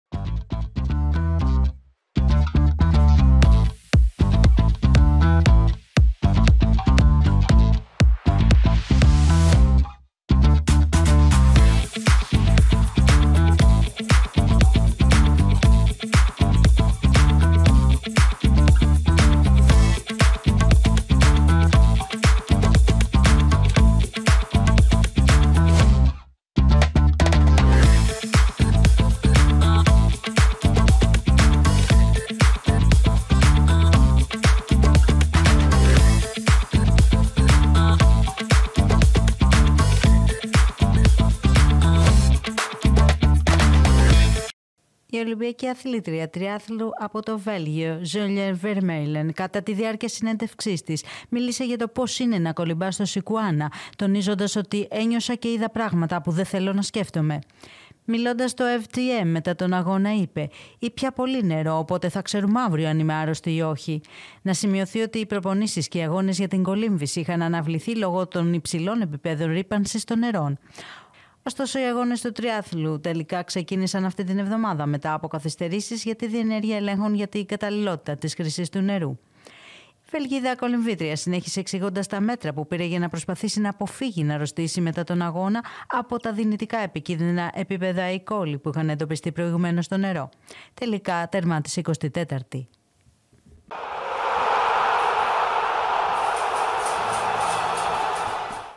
Αθλήτρια τριάθλου για Σηκουάνα: «Ένιωσα και είδα πράγματα που δεν θέλω να σκέφτομαι» | Pronews TV
Η αθλήτρια τριάθλου από το Βέλγιο Jolien Vermeylen, κατά τη διάρκεια συνέντευξής της, μίλησε για το πώς είναι να κολυμπάς στον Σηκουάνα, τονίζοντας πώς «ένιωσα και είδα πράγματα που δεν θέλω να σκέφτομαι».

Μιλώντας στο VTM μετά τον αγώνα, είπε: «Ήπια πολύ νερό, οπότε θα ξέρουμε αύριο αν είμαι άρρωστη ή όχι» και συνέχισε:

«Δεν έχει γεύση Coca-Cola ή Sprite, φυσικά. Καθώς κολυμπούσα κάτω από τη γέφυρα, ένιωσα και είδα πράγματα που δεν πρέπει να σκεφτόμαστε πολύ. Ο Σηκουάνας ήταν βρώμικος εδώ και εκατό χρόνια, οπότε δεν μπορούν να πουν ότι η ασφάλεια των αθλητών είναι προτεραιότητα. Αυτά είναι βλακείες!».

Να σημειωθεί ότι οι προπονήσεις και οι αγώνες για την κολύμβηση είχαν αναβληθεί λόγω των υψηλών επιπέδων ρύπανσης των νερών.

Ωστόσο, οι αγώνες του τριάθλου τελικά ξεκίνησαν αυτή την εβδομάδα μετά από καθυστερήσεις για τη διενέργεια ελέγχων για την καταλληλότητα της χρήσης του ποταμού

Τι μέτρα πήρε πριν βουτήξει στον Σηκουάνα

Η Vermeylen συνέχισε εξηγώντας τα μέτρα που πήρε για να προσπαθήσει να αποφύγει να αρρωστήσει μετά τον αγώνα από τα δυνητικά επικίνδυνα επίπεδα E.Coli που είχαν εντοπιστεί προηγουμένως στο νερό.

«Αν ο αγώνας δεν είχε γίνει, θα ήταν ντροπή για τον οργανισμό, για το Παρίσι, για τη Γαλλία. Ήταν τώρα ή ποτέ, και δεν μπορούσαν να ακυρώσουν εντελώς τον αγώνα.